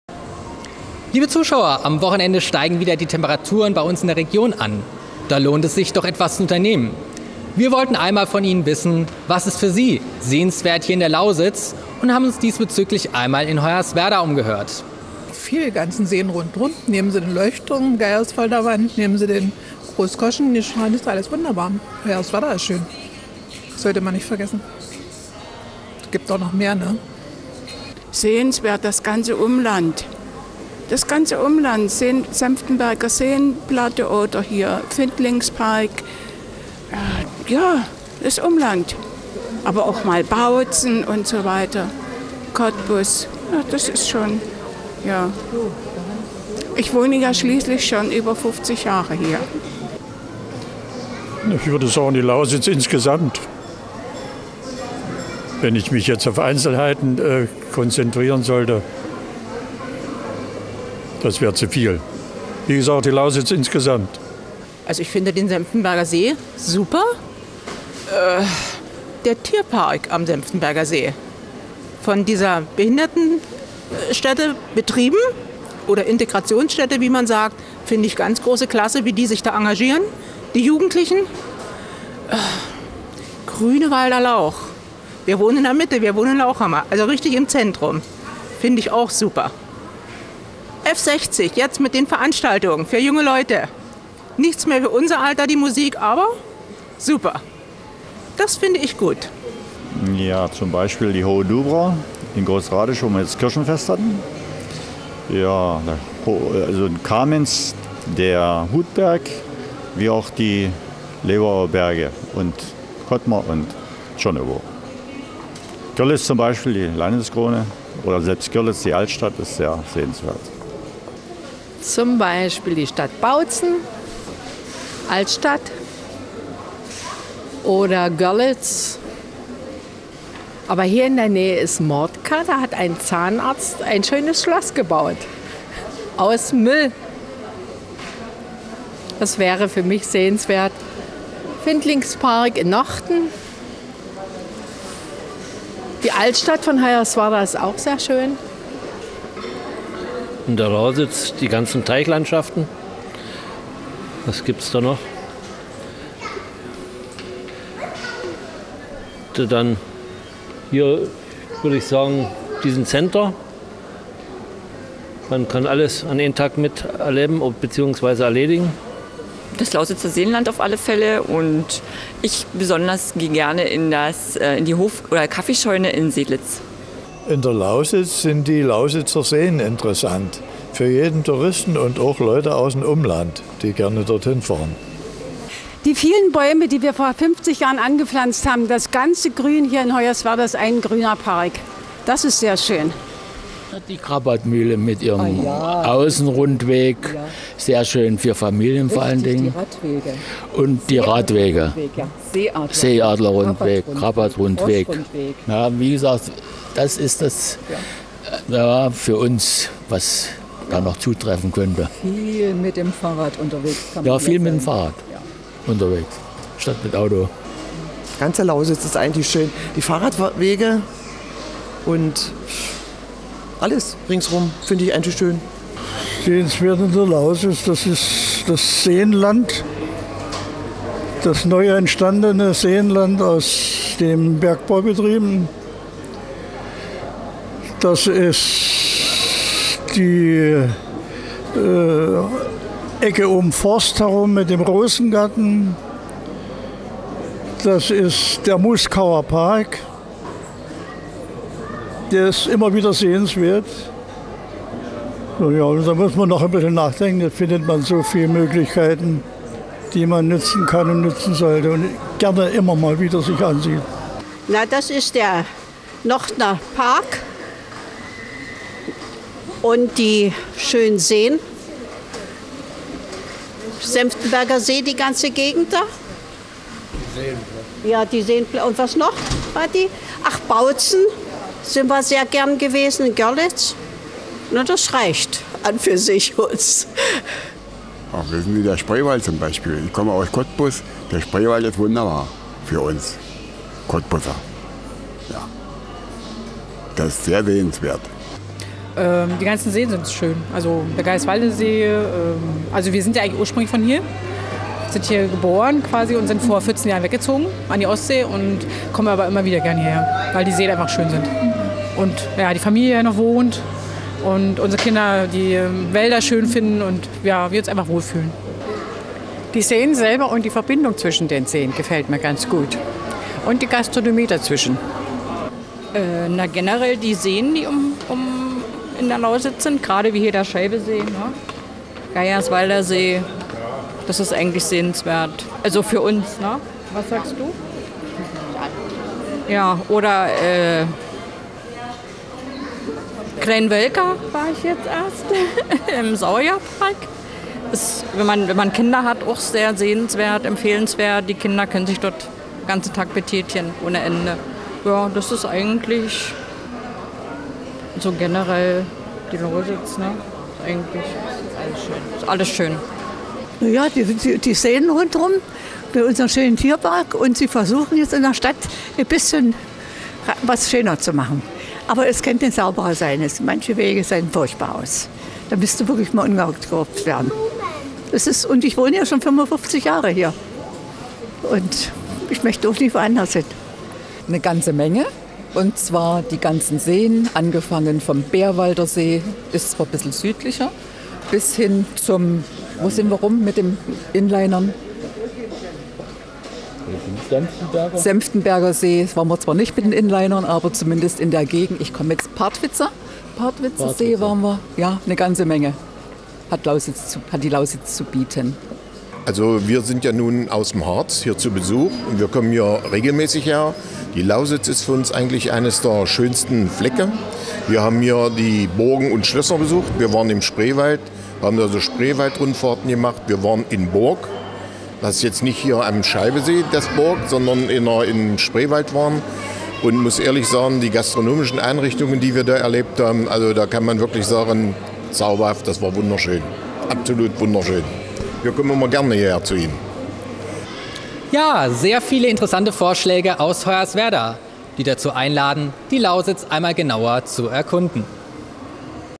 Umfrage: Sehenswertes in der Lausitz - LAUSITZWELLE
Am Wochenende steigen die Temperaturen in der Region wieder an. Da lohnt es sich doch etwas zu unternehmen. Wir wollten einmal wissen, was für Sie in der Lausitz sehenswert ist und haben uns in Hoyerswerda umgehört. (red)